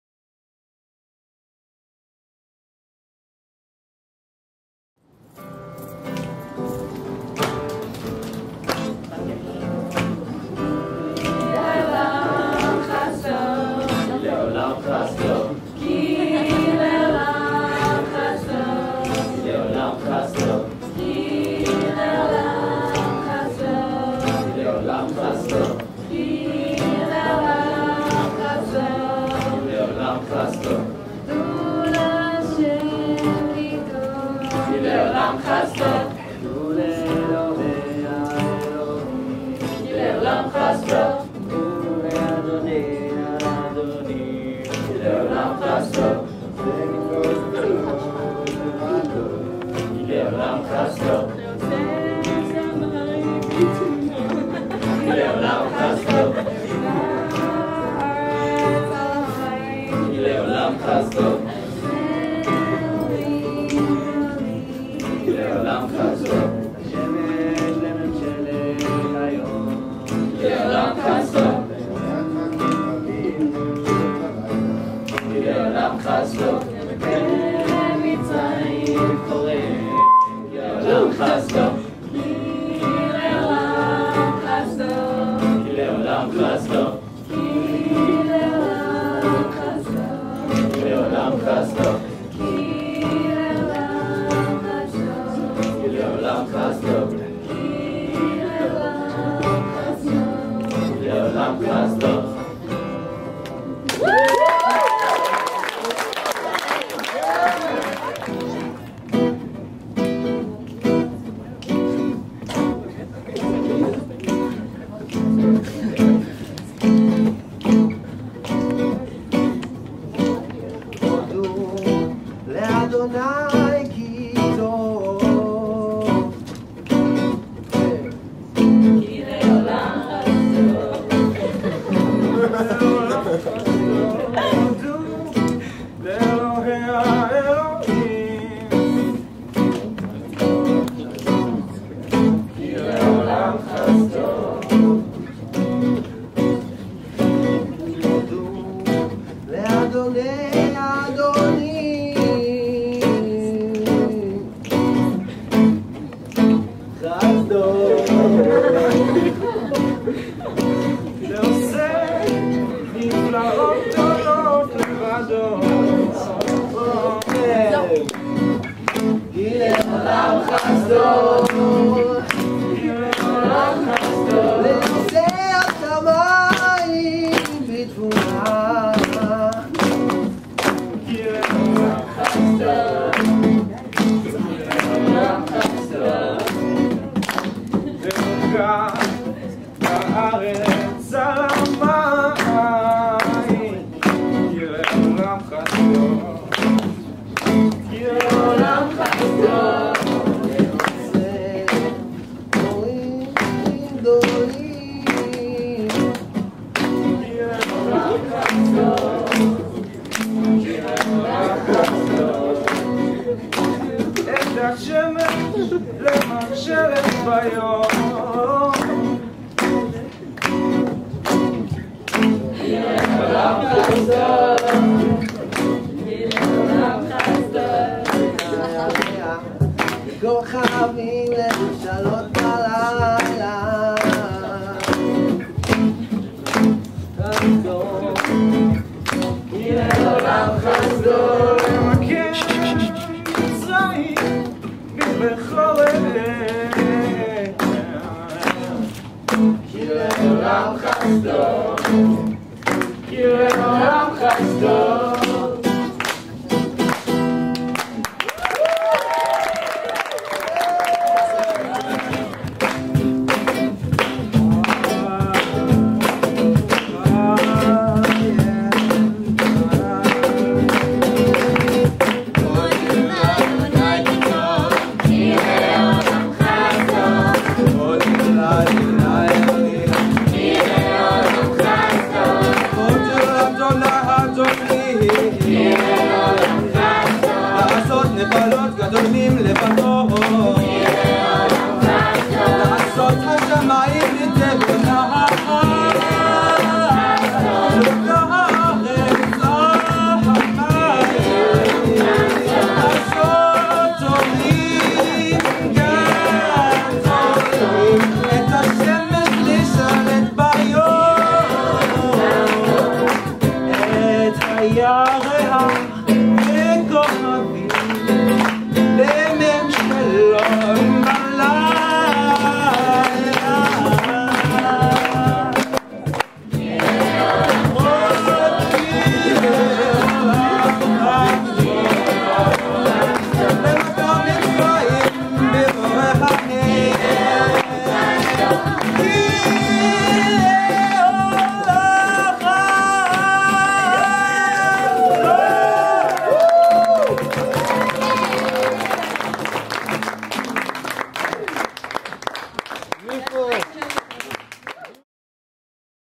Mifgashim, Young Judaea Year Course Arts Track Tehillim with Roi Levi
Young Judaea Year Course Arts Track performing their original tehillim - Program by Roi Levi at WOMINI (World Music Institute of Northern Israel) Shorashim, Misgav, Galilee